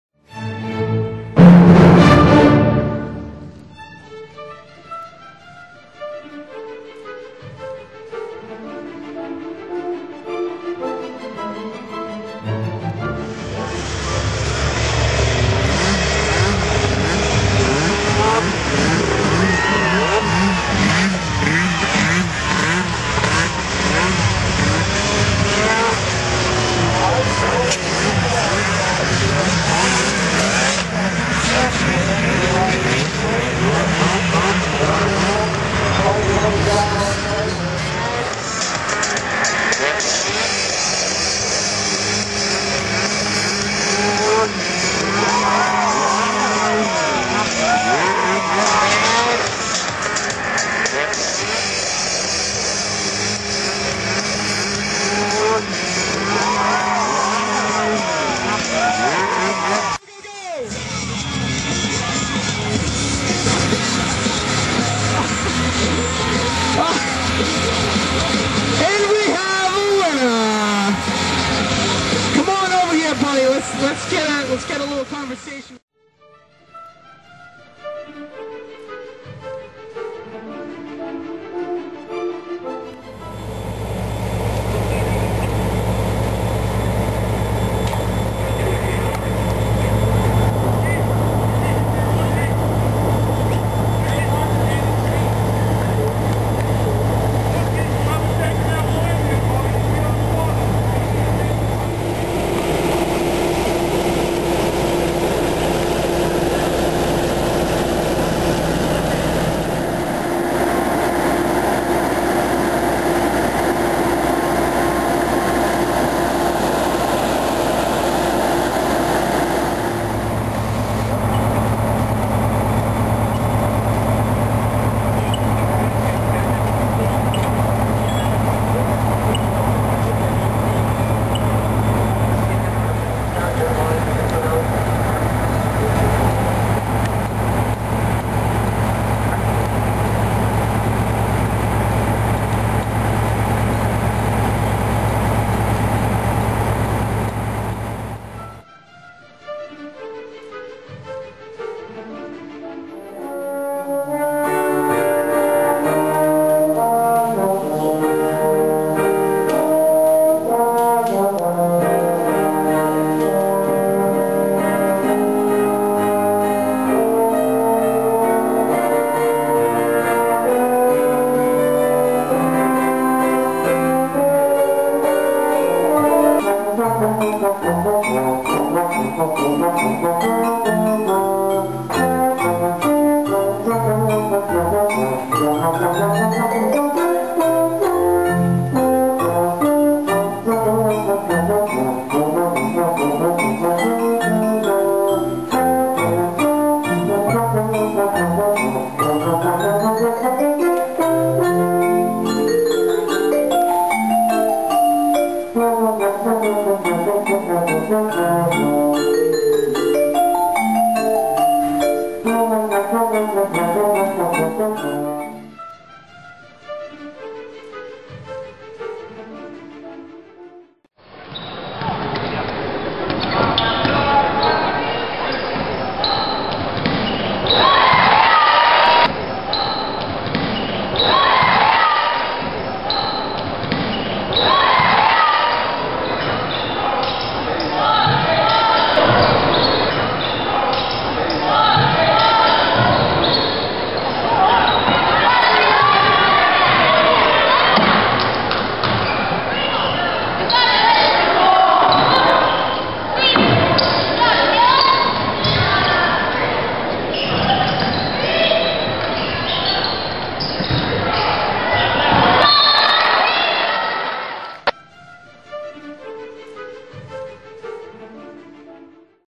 February 26 2010 Video News of the Week.wmv
Featuring fire at a home in East Hereford,Quebec; Sno-X races in Groveton; music at the Coos County Nursing Hospital; Groveton girls Class S semifinals.